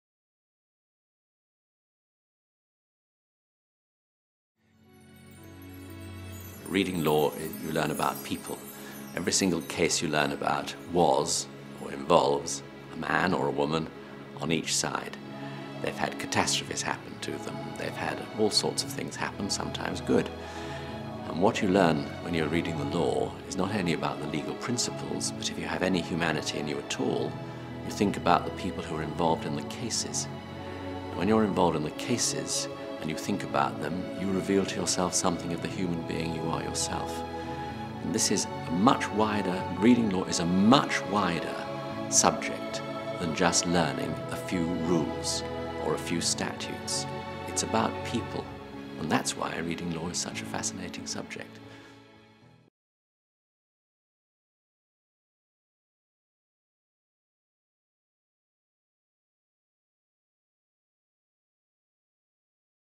Lord Judge on 'Why study law?'
Rt Hon Lord Judge, Distinguished Visitor and Visiting Professor in The Dickson Poon School of Law, King's College London on 'Why study law?'.